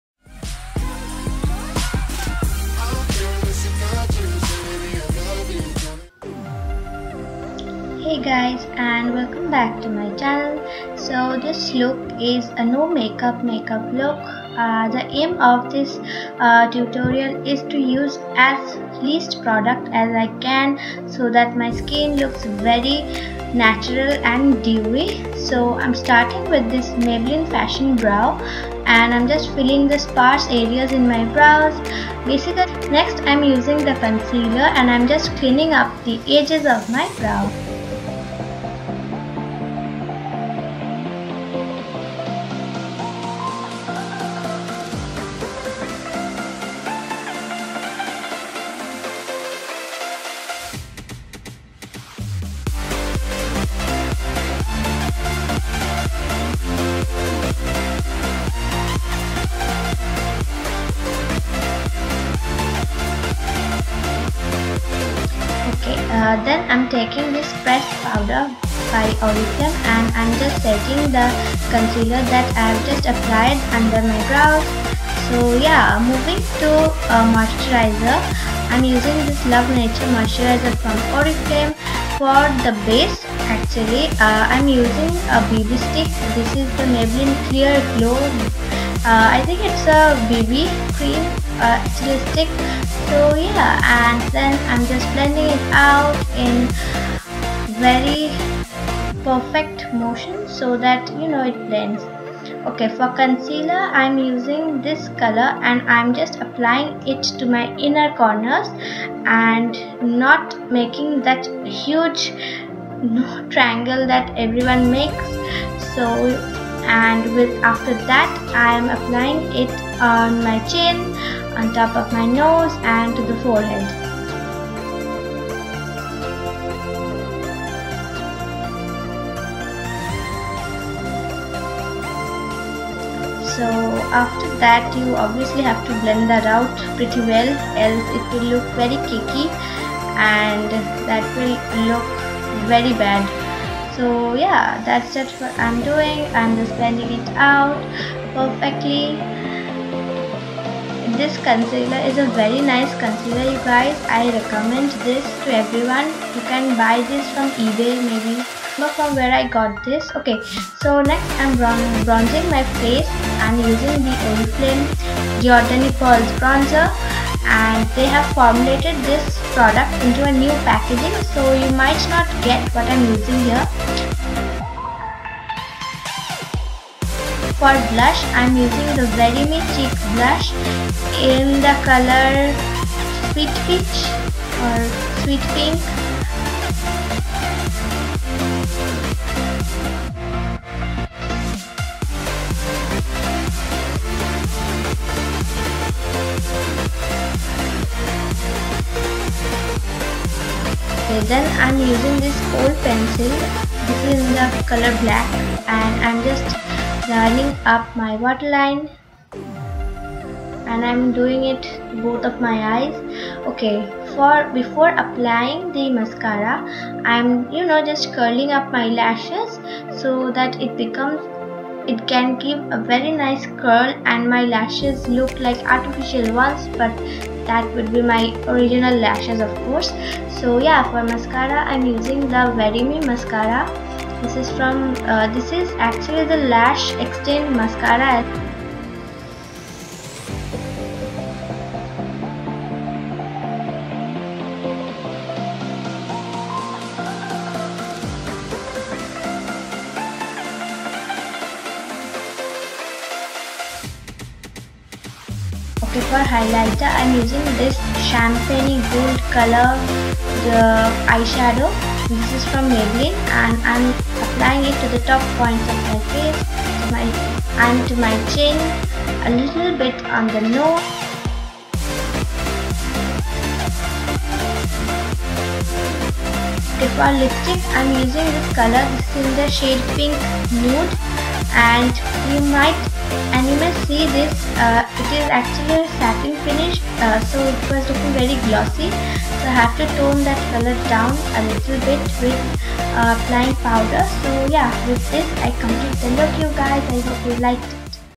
NO MAKEUP MAKEUP TUTORIAL || NATURAL LOOKING MAKEUP || GIVEAWAY (check description box)
Hey guys today's video is on a no makeup makeup look . Since most of you guys really prefer very natural looking skin over full coverage makeup so I created this look. :}

**Also I am planning to have a giveaway once i reach a 100 subscribers.

PRODUCTS USED :
Maybelline fashion brow duo stick
Camouflage 12 colour concealer
Oriflame pressed powder
Oriflame kohl pencil
Veryme lash extend mascara
Maybelline clear glow bb stick
Giordani bronzing pearls (natural radiance)
Veyme blush (Sweet coral)
The studio artist lipstick (pink nude)
Maybelline diamond glow eyeshadow palette